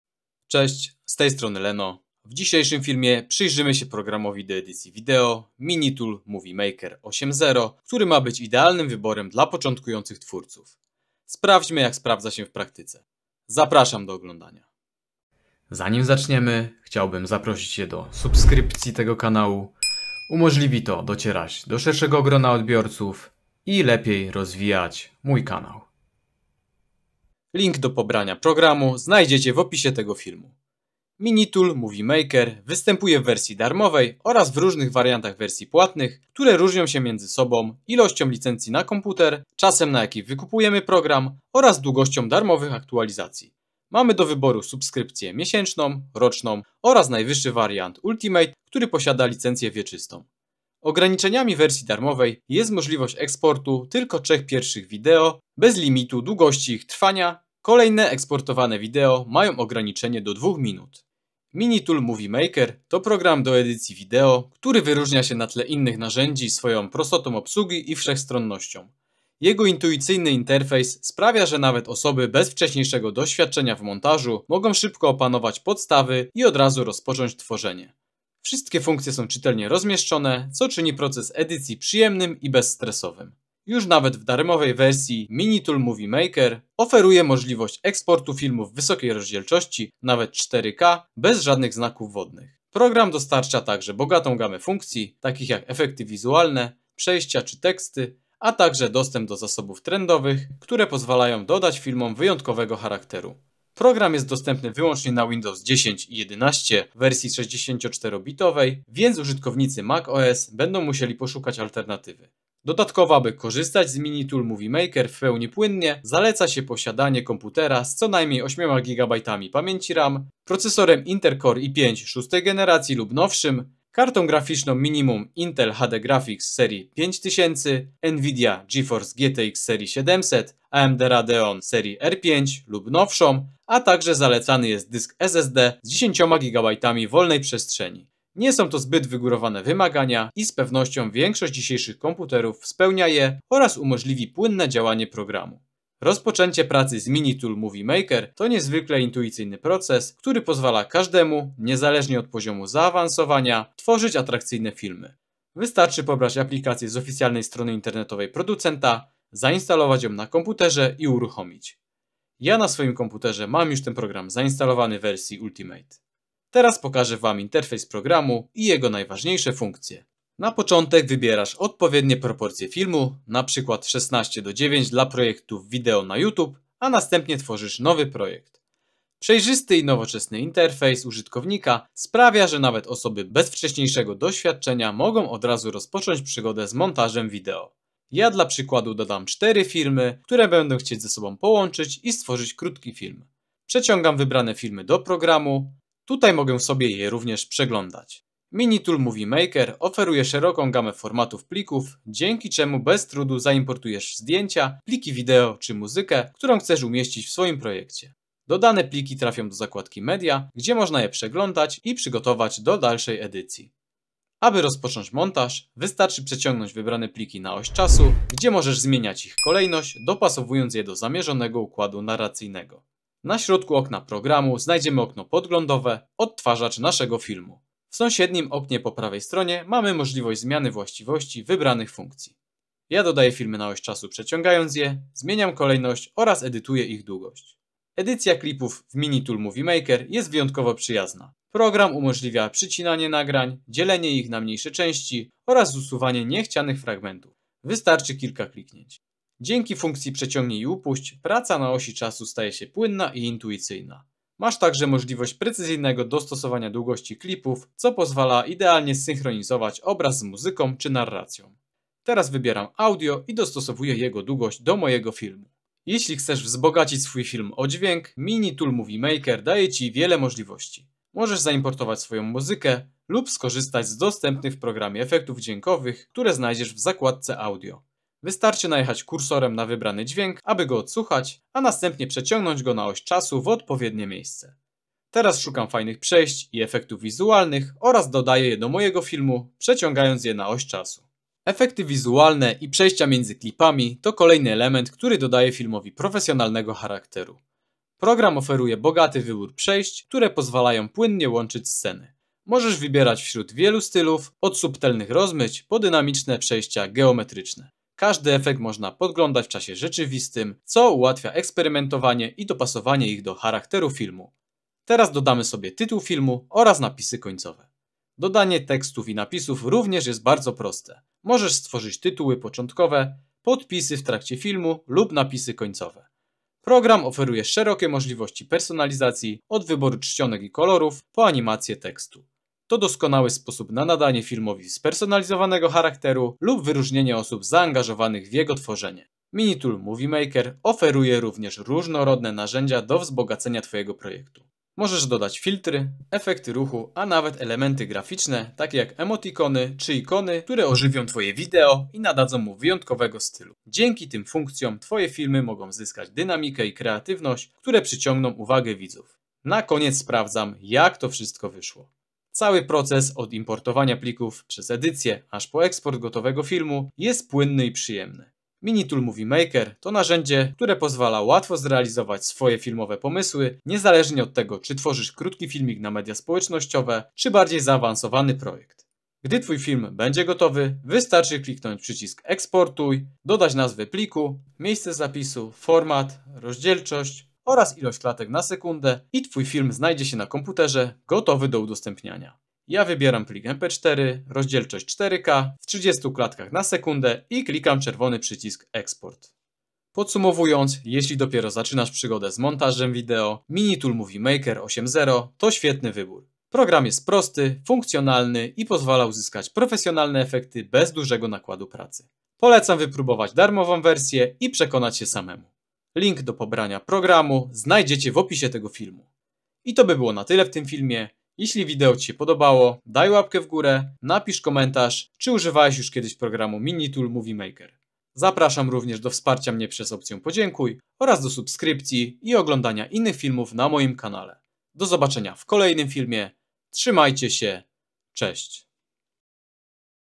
Najlepszy program do edycji wideo dla początkujących - MiniTool MovieMaker 8.0
W dzisiejszym filmie pokażę najważniejsze funkcje programu MiniTool MovieMaker 8.0. Czy to najlepszy program do edycji wideo dla początkujących? Zapraszam do oglądania i subskrypcji kanału.

••• 💻 Śledź mnie w swoich ulubionych mediach społecznościowych! •••